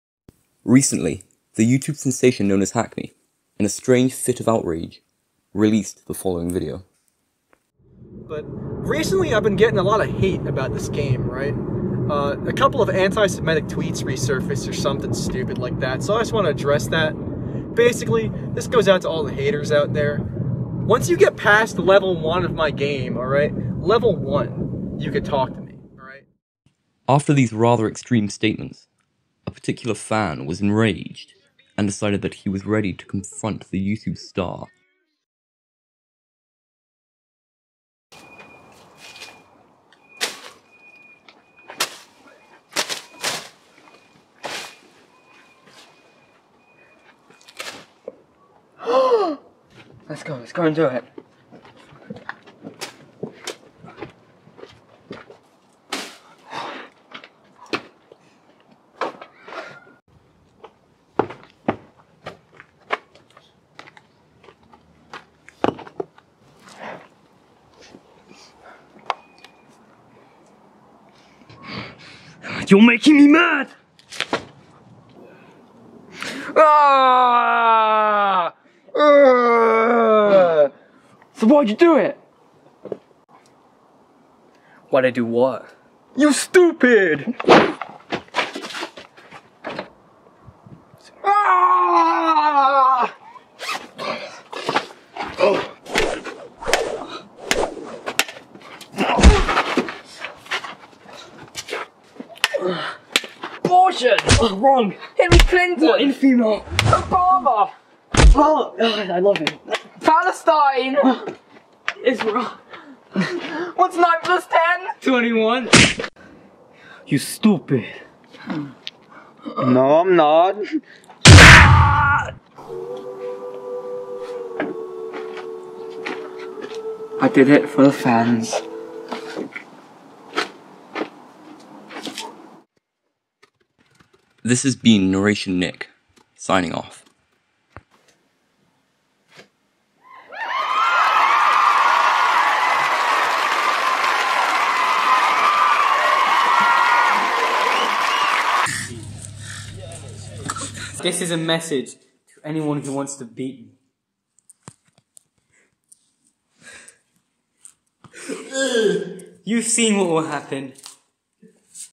HACKMI IS CONFRONTED BY THE FANS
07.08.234DN. An immensely angry Hackmi fan is taking the law into his own hands. This won't be good for his reputation. What will the public think???
Credits:
DAN WHITE
Hackmi? (nah we got a double)
White kid (narrator nick)
mchad001
depression

Actual credits:
Hackmi
Will
Filmed by Rhys.W
mccad001
dude ur metroid stuff was awesomer than this